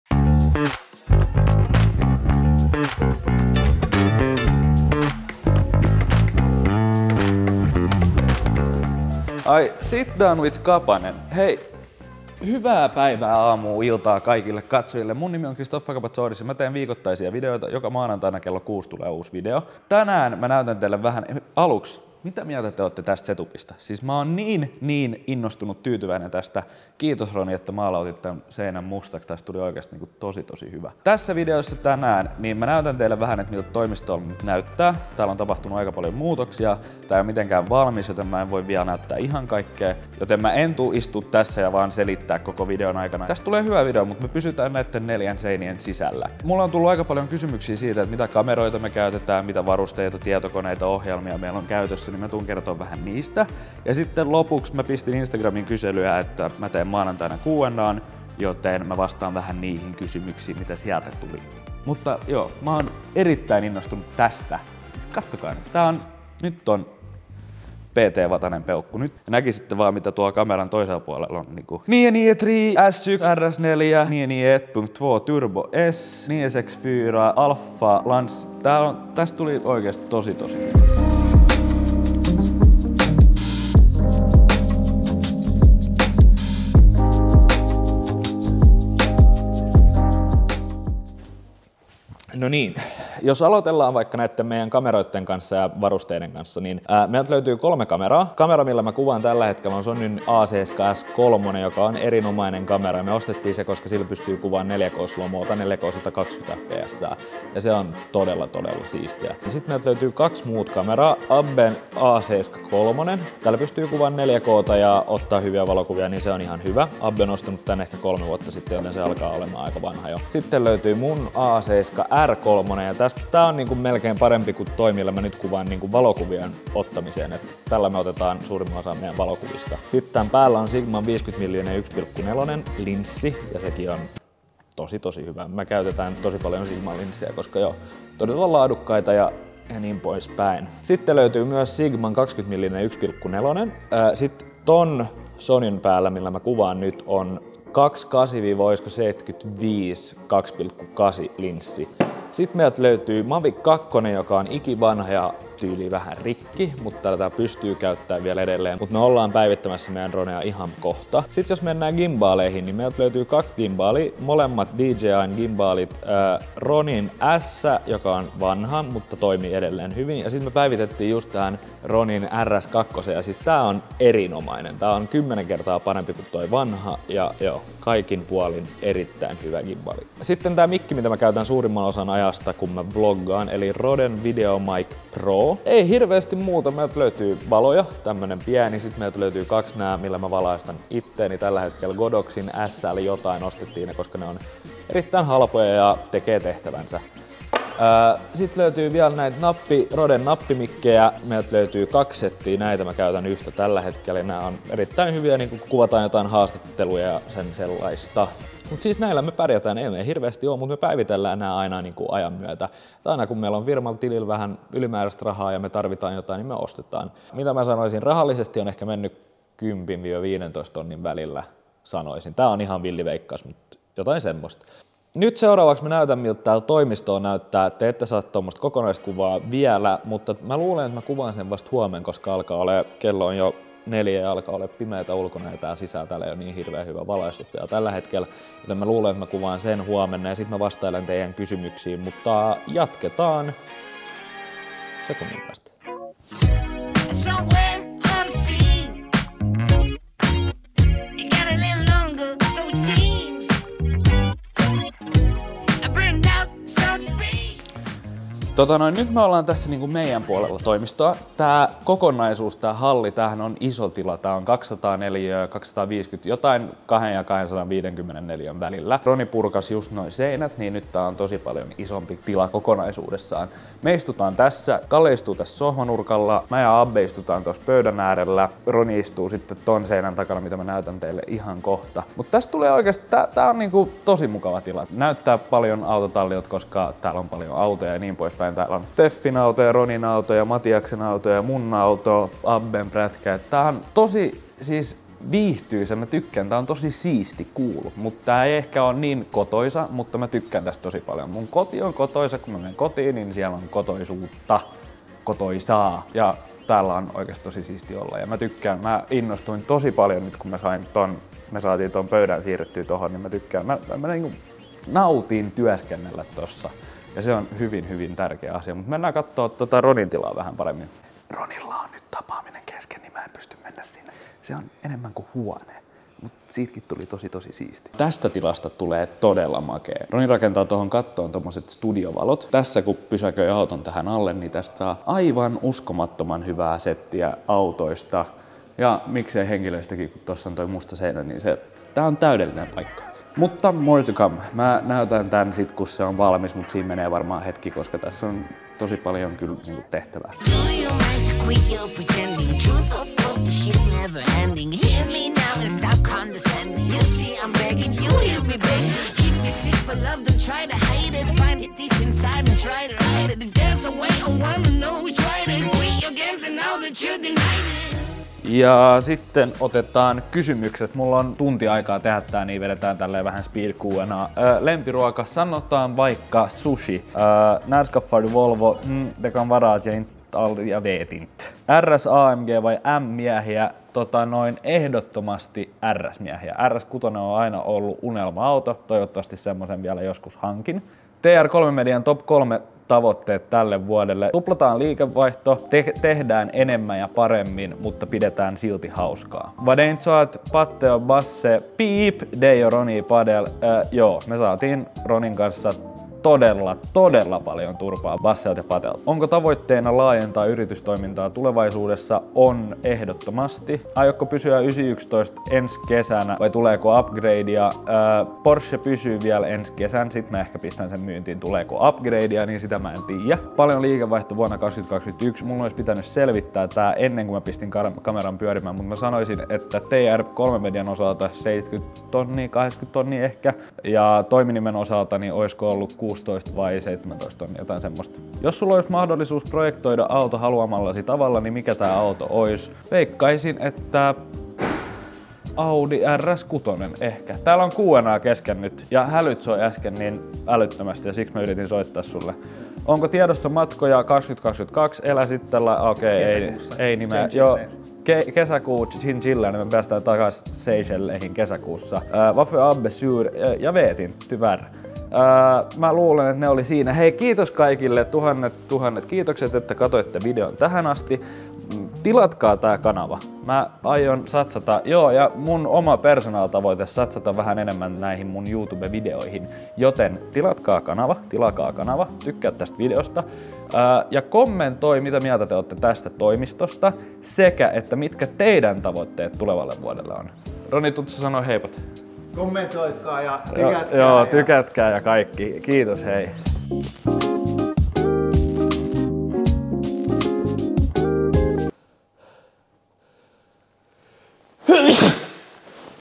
LIIKEVAIHTO 2021!? - Vlog #45
Tässä videossa käydään läpi mitkä kamerat meillä on käytössä. Vastailen teidän kysymyksiin, sekä näytän miltä TOIMISTO 2.0 Näyttää.

Kalusto:
Sigma: 20mm 1.4
Sigma: 50mm 1.4
Tamron: 28-75mm 2.8